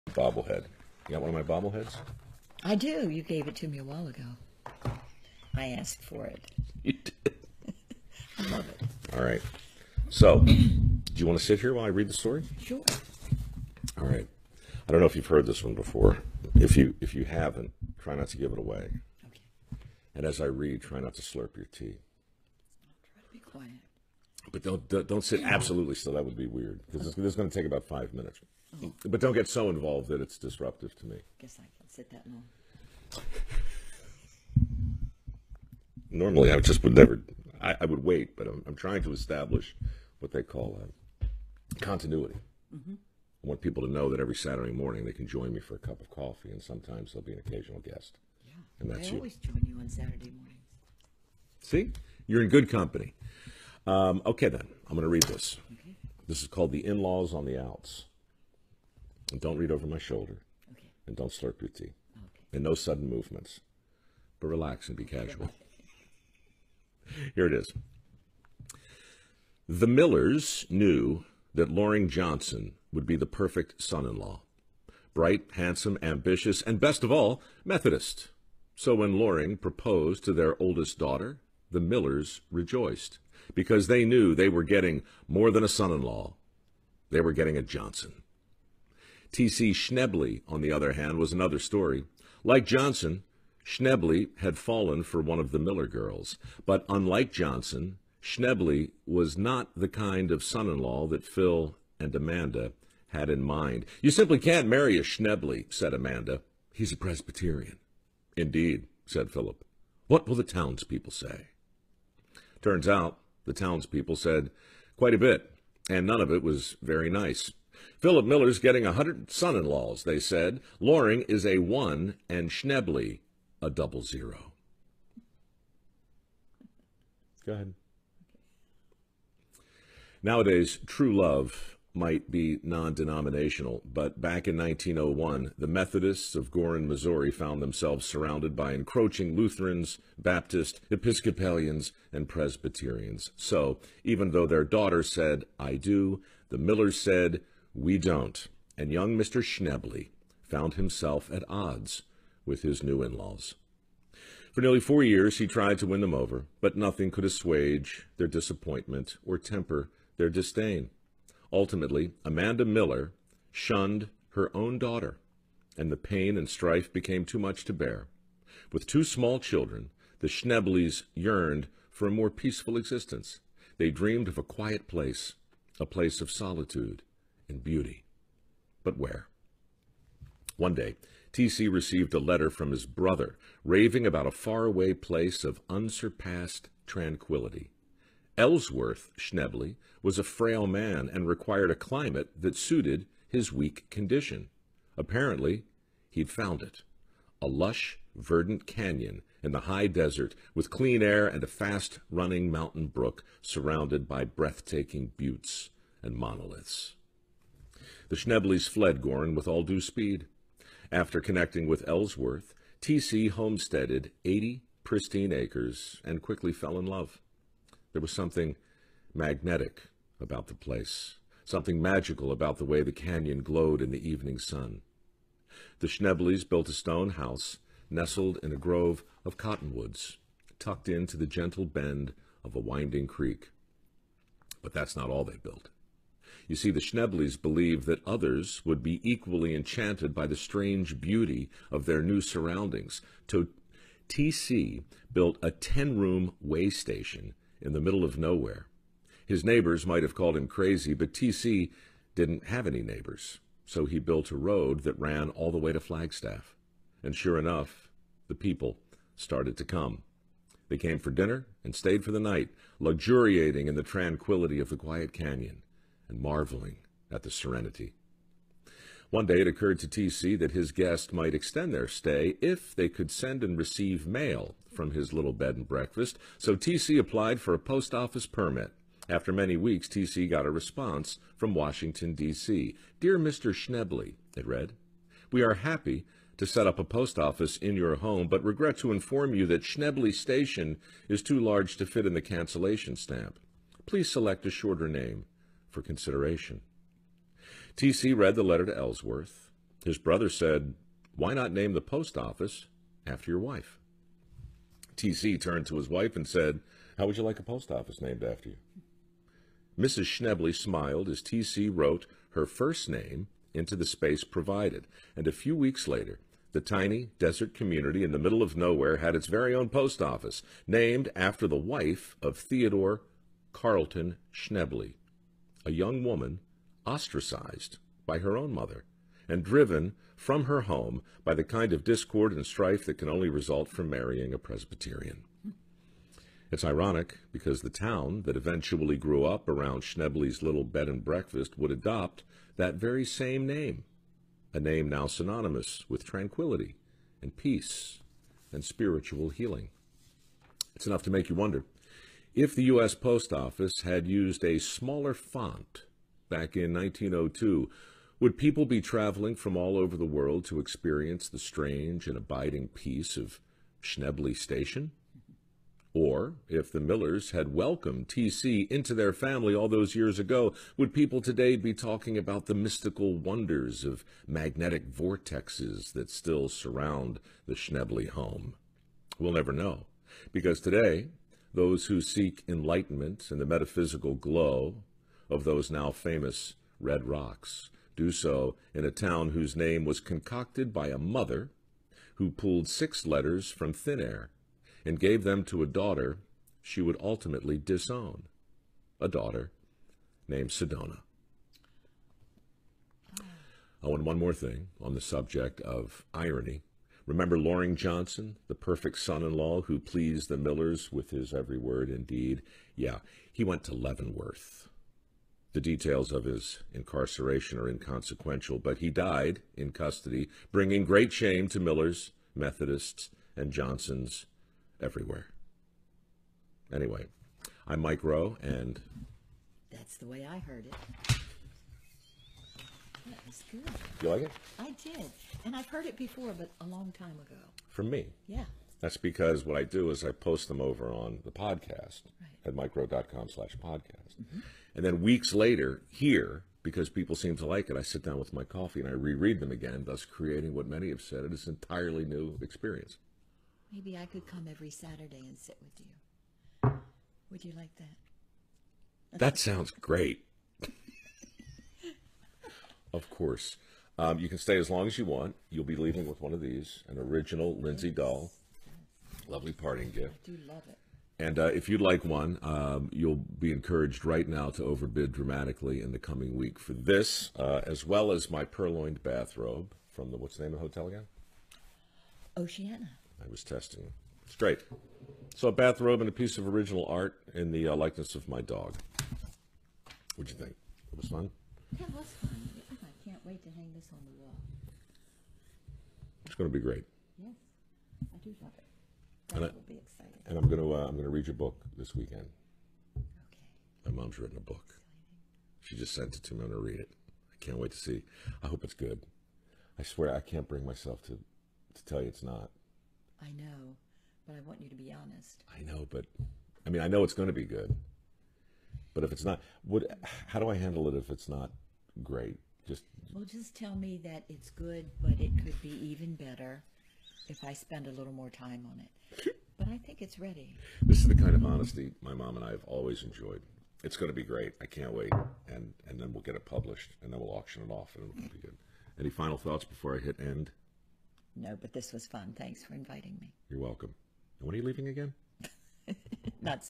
Mike Rowe with his Mom reading a really nice letter
Love is the strangest thing and this story from Mike Rowe very interesting as well:-) Enjoy nd thank you to Mike Rowe for sharing this.